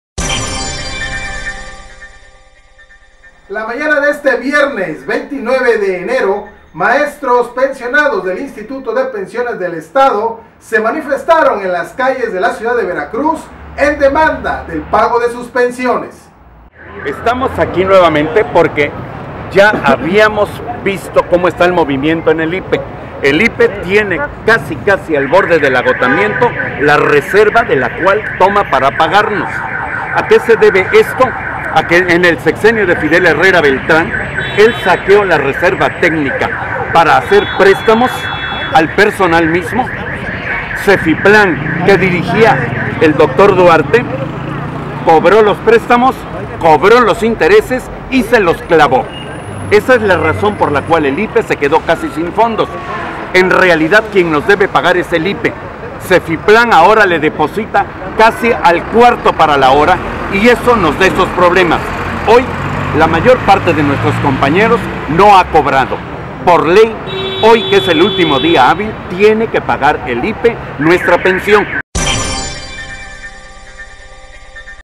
Maestros pensionados del IPE toman calles de Veracruz
Ante la falta de pago, maestros pensionados del IPE tomaron calles del centro de la Ciudad de Veracruz